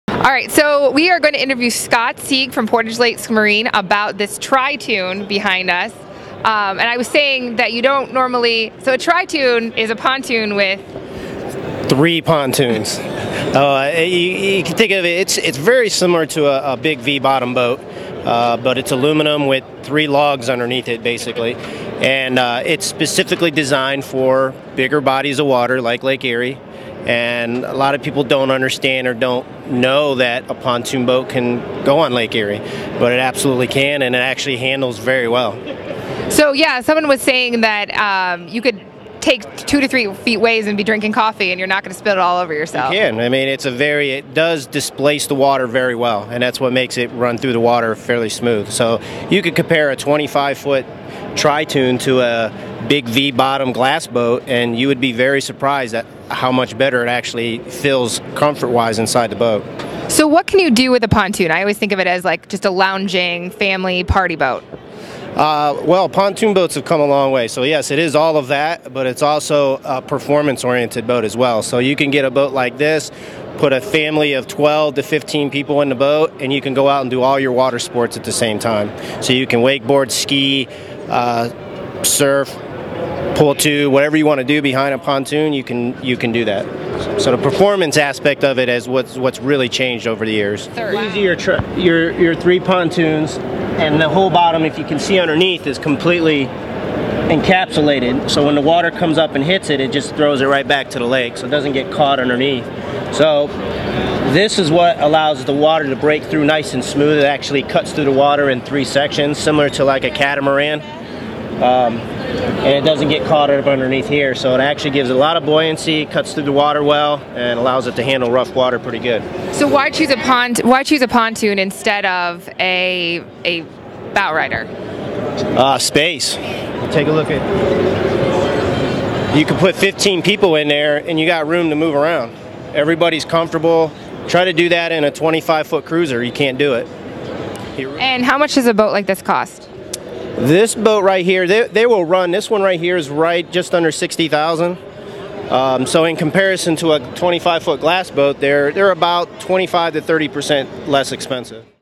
How is tritoon boat different than a pontoon boat?
Take a look at a tritoon, and see how it's vastly improved over the pontoons from years ago.  Rock the Lake's Laura Johnston finds out why a tritoon is a great option for Lake Erie as she explores the Mid-America Boat Show and fish now expo at the IX Center in Cleveland.